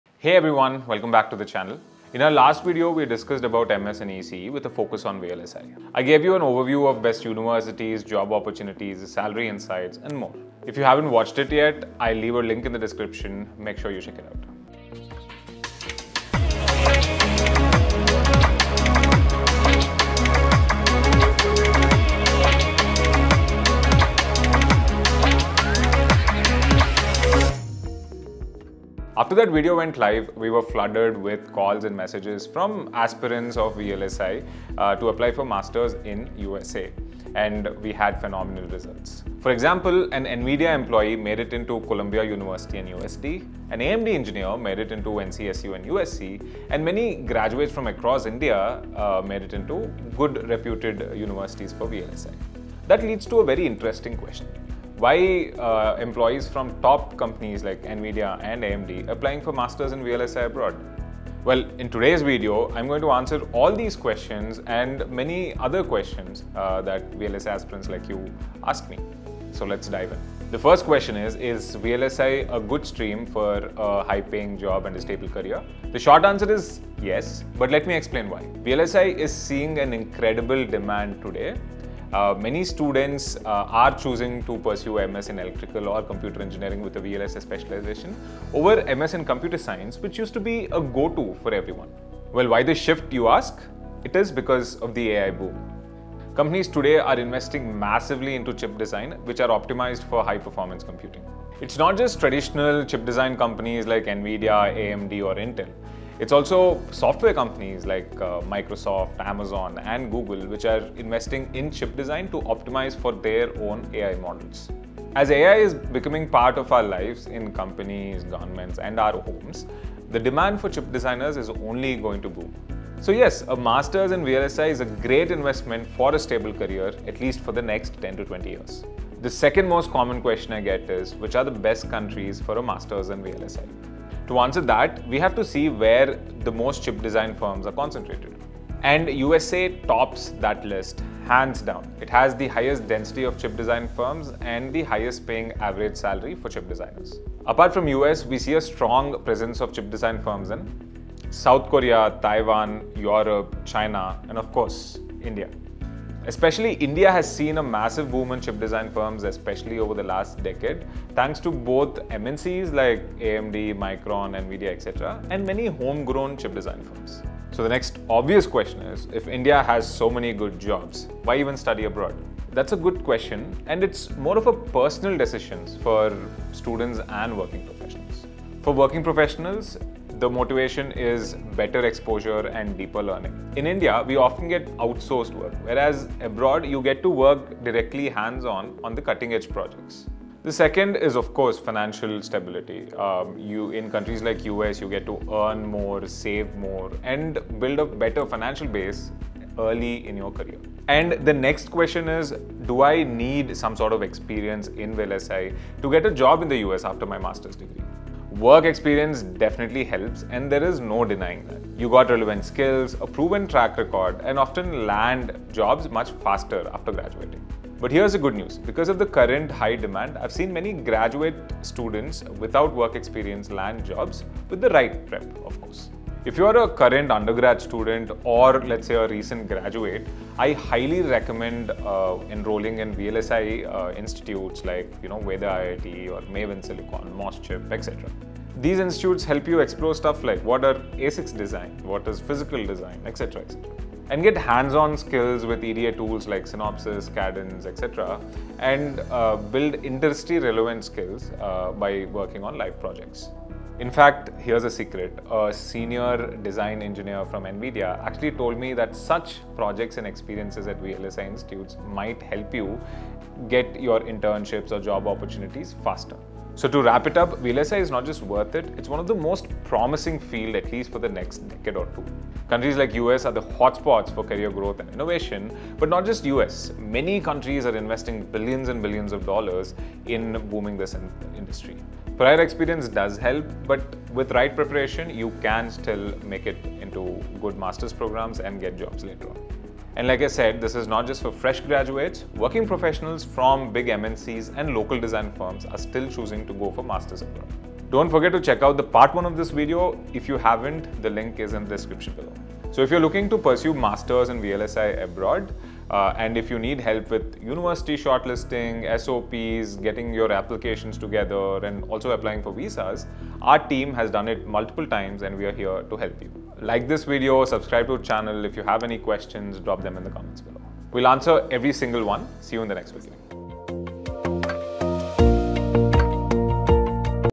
Is Masters degree in VLSI worth it ? Answering your most common questions | Part 2
Is a Master’s in VLSI really worth it in 2025? In this follow-up video, we answer the most common questions VLSI aspirants have - from salary prospects and global job demand to whether prior work experience is required. You’ll find out why even working professionals from companies like NVIDIA and AMD are opting for a Master’s abroad, and how students from India are landing admits in top U.S. universities like Columbia, UCSD, NCSU, and USC. We also explore how the rise of AI has fueled a global chip design boom, making VLSI one of the most promising and future-proof fields in tech today. If you're confused between Computer Science and VLSI, or wondering where to study, how to prepare, and whether you can get a job after MS without experience, this video will give you the clarity you need.

Whether you're a college student or working in the industry, this is your guide to making an informed decision about pursuing VLSI abroad.

For expert help with MS applications (SOPs, shortlisting, interviews), reach out to our team today.